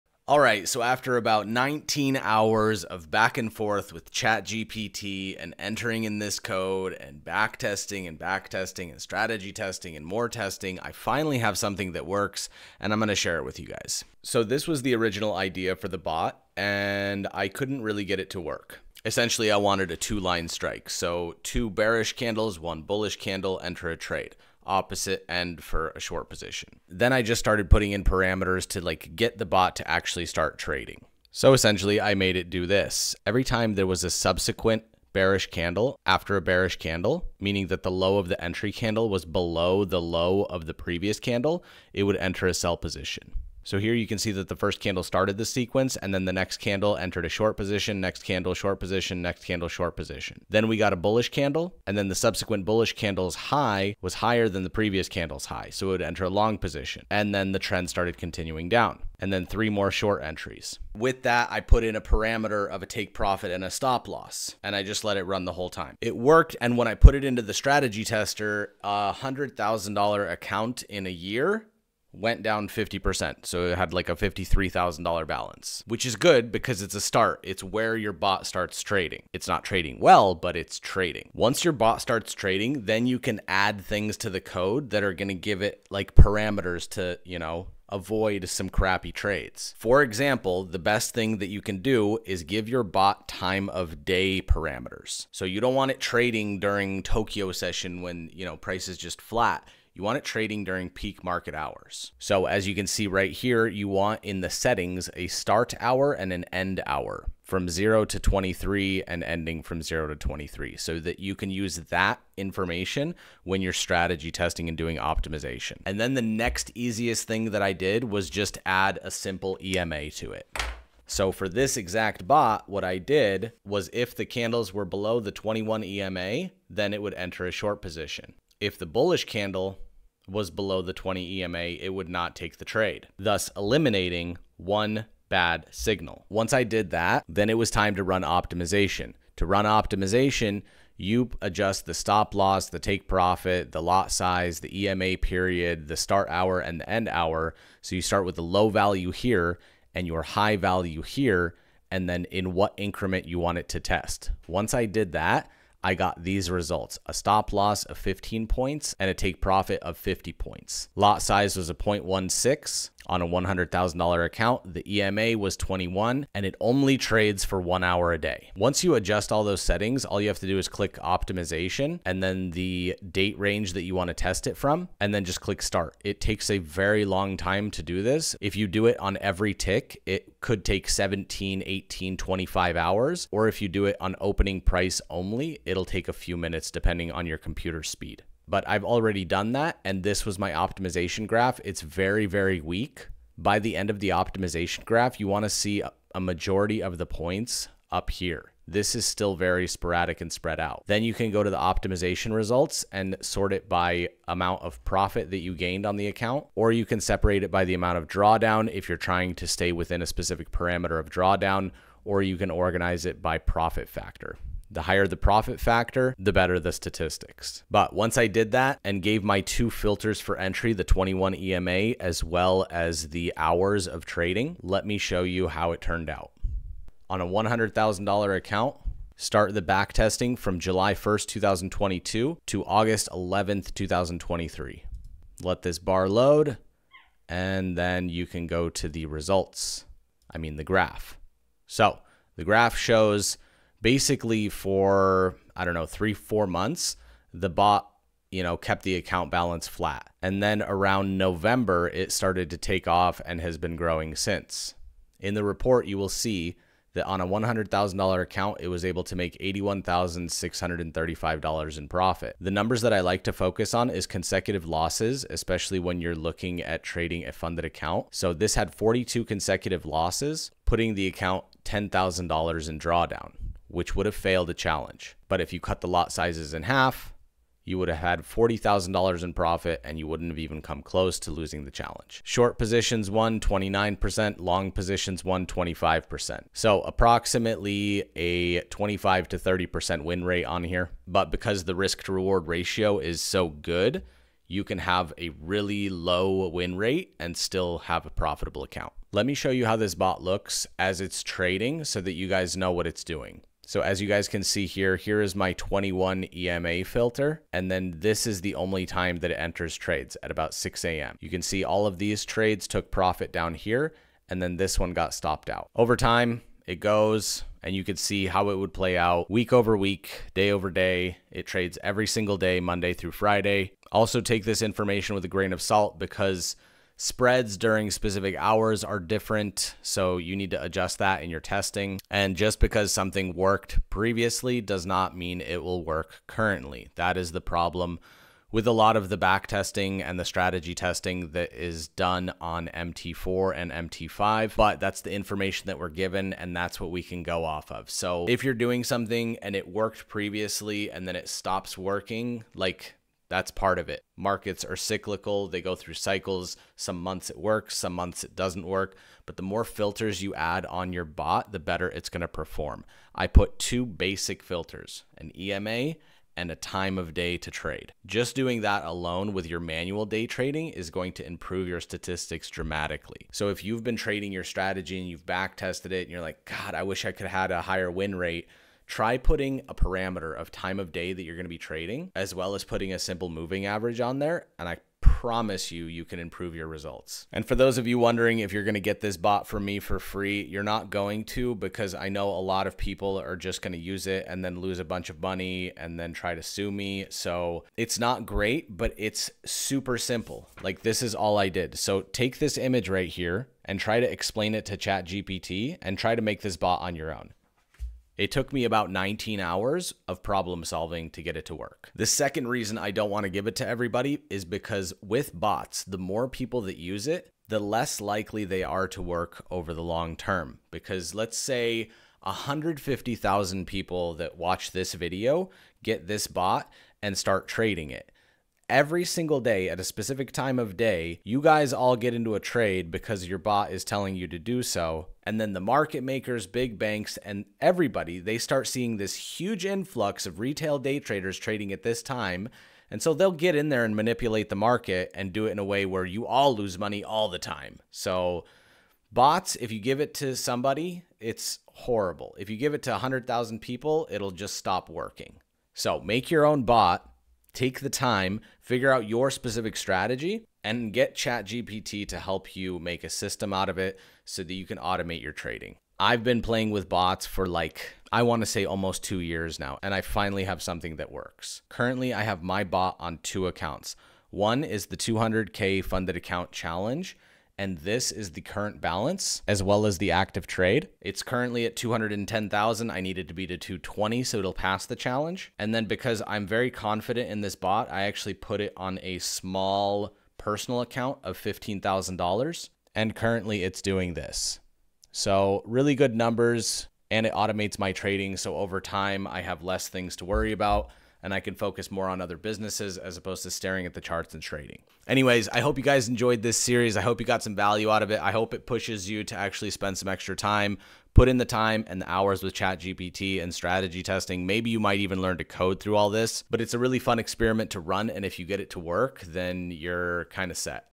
How to Make a Trading Bot Part 3
part 3. In this video, I use artificial intelligence and ChatGPT to write a bot in the meta-trader trading platform coding language. This will be a process of how you would take your rules based strategy and turn it into an automated system the trades for you to generate yourself some passive income.

if you want in depth day trading education as well as a community and custom buy sell indicators join the trading floor today